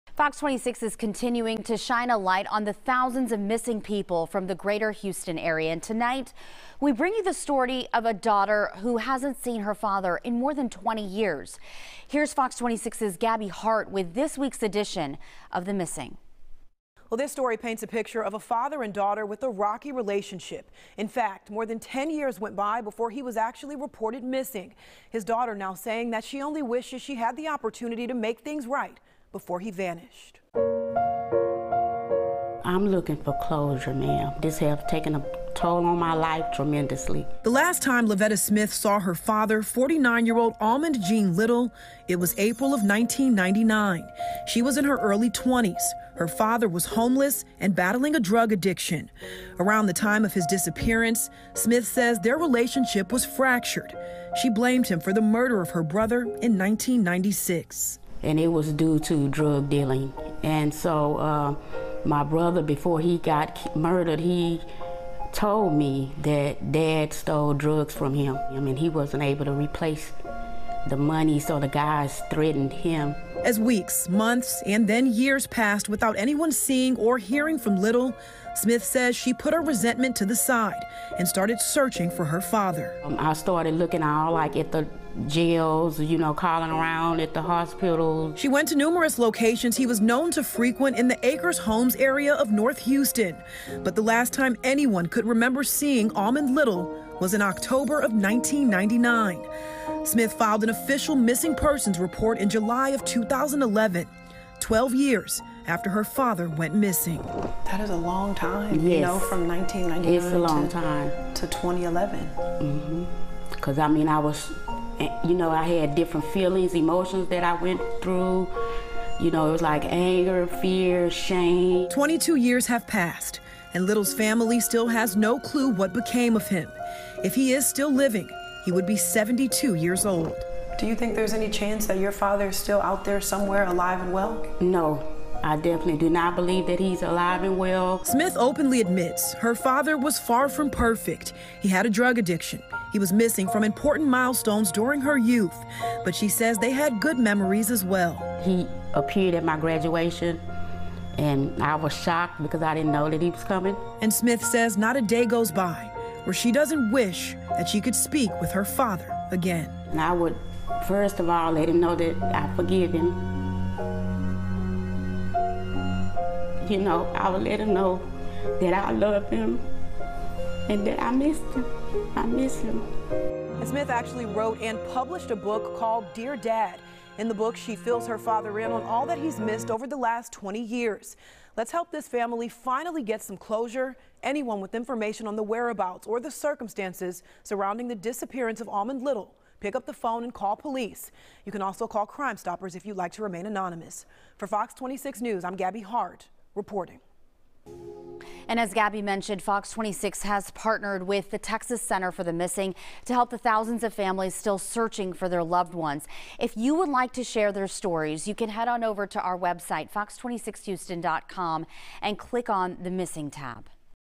Missing Almond Gene Little from Houston last seen more than 20 years ago
A daughter says the last time she saw her father, 49-year-old Almond Gene Little, was April 1999.